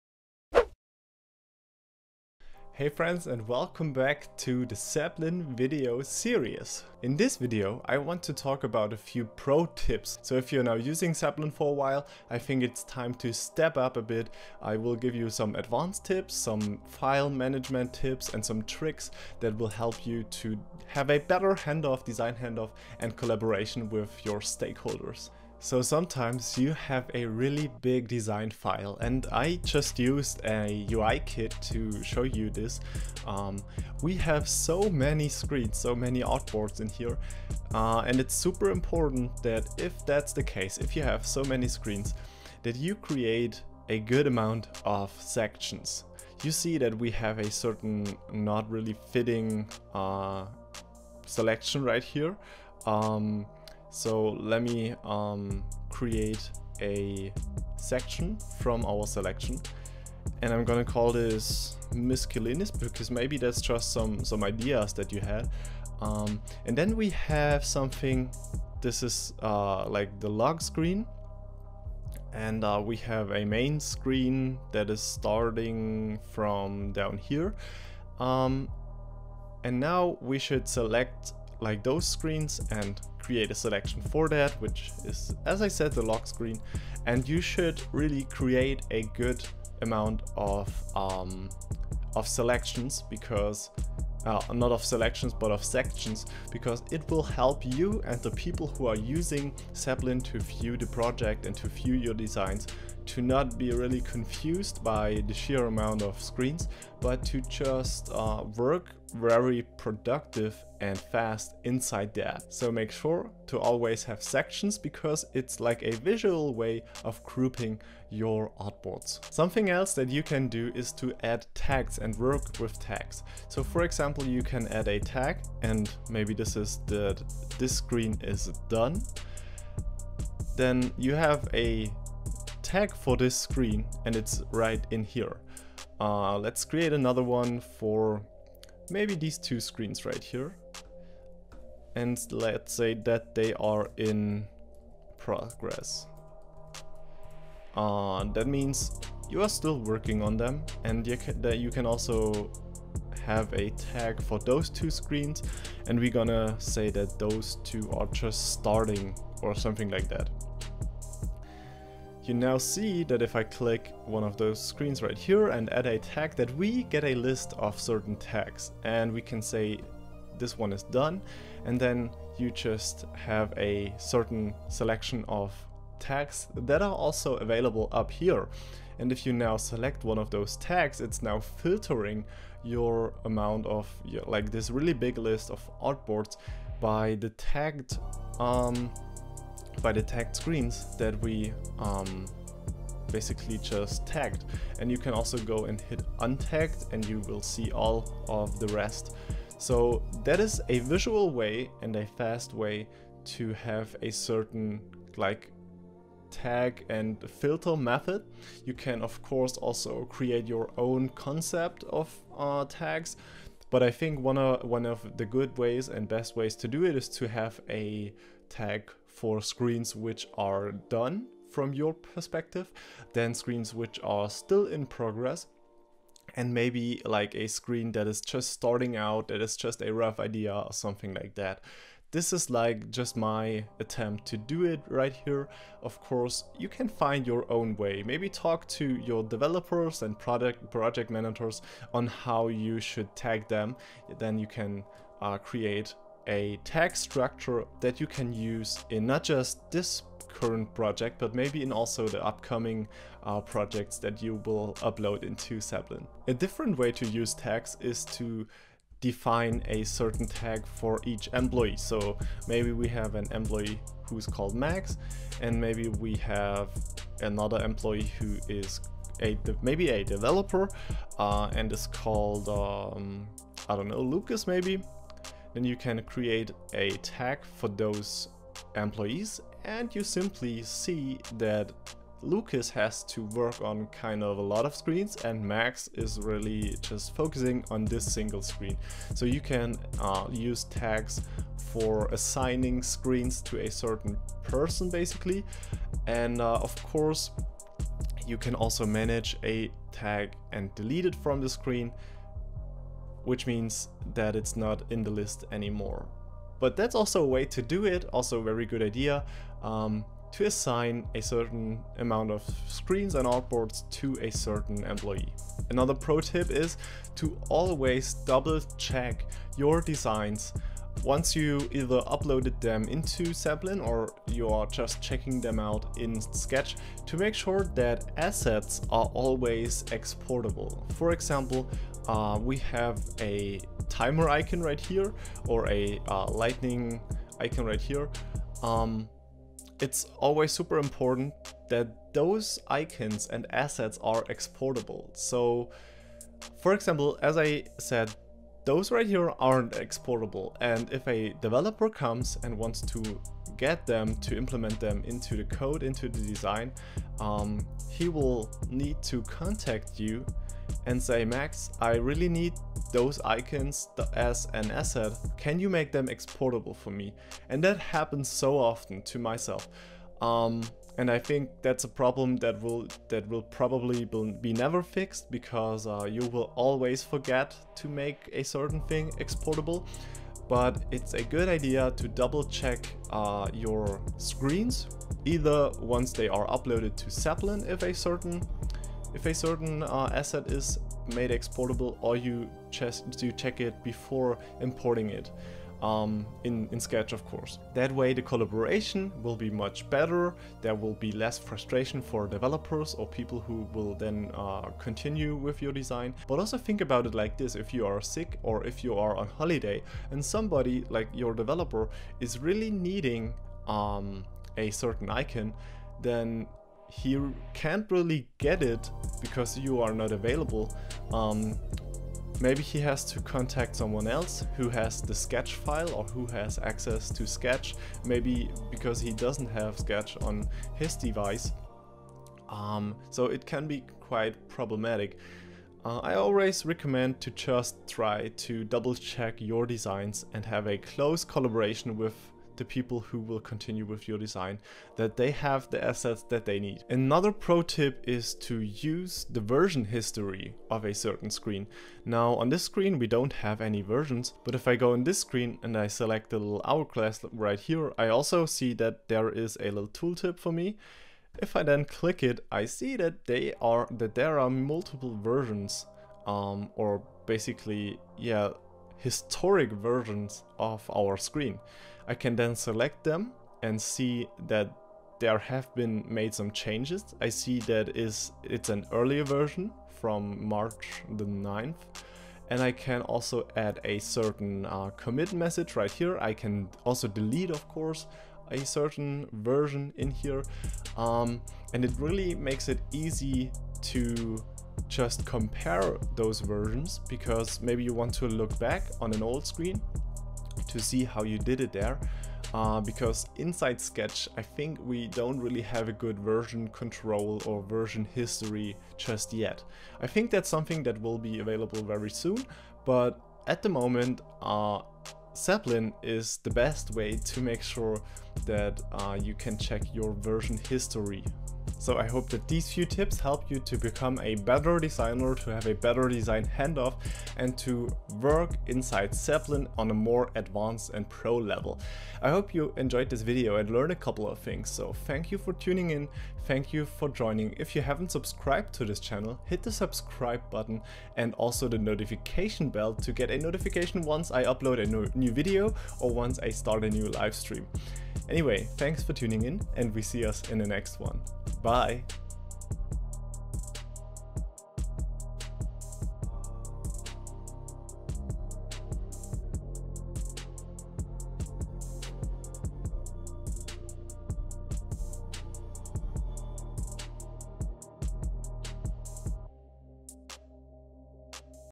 Zeplin Basics: Pro Tips for better Project Management, Workflows & Productivity
Zeplin is a Mac and Windows tool that helps designers, developers and product managers managing and maintaining user interface designs as well as creating better hand offs and multiple design systems. Zeplin comes with a variety of features and can import Sketch and Figma files.

MY GEAR